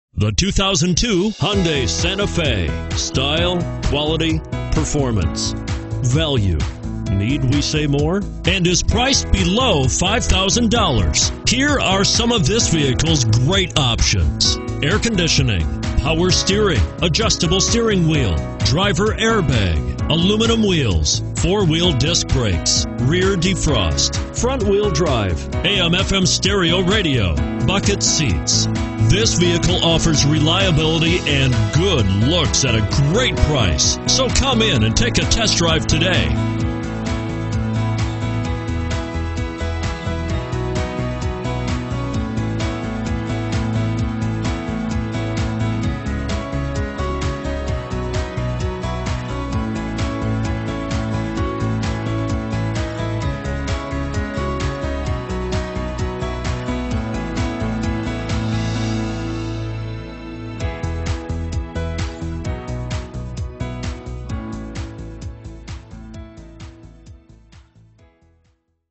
2002 HYUNDAI SANTA FE Arlington, TX 2U299100
2002 HYUNDAI SANTA FE Arlington, TX
Stock# 2U299100

AutoNation Toyota North Arlington (USED)
1661 Wet N' Wild Way

Pick up this Crystal Blue 2002 Hyundai Santa Fe, available today. Featuring Automatic transmission, it has 176453 miles. This could be the one you've been searching for.